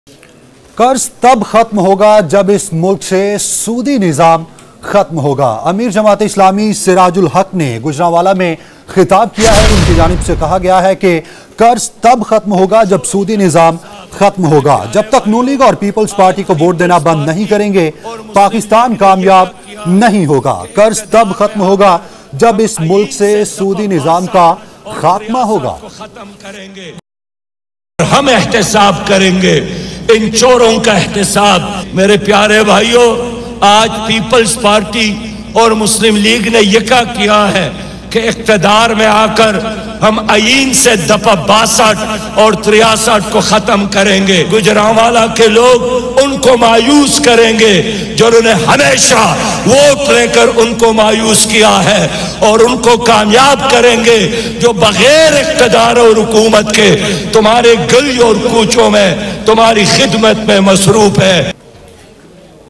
Siraj Ul Haq lashes out At Nawaz Sharif and PPP | Express News
Latest News from Gaza March | Siraj Ul Haq | Express News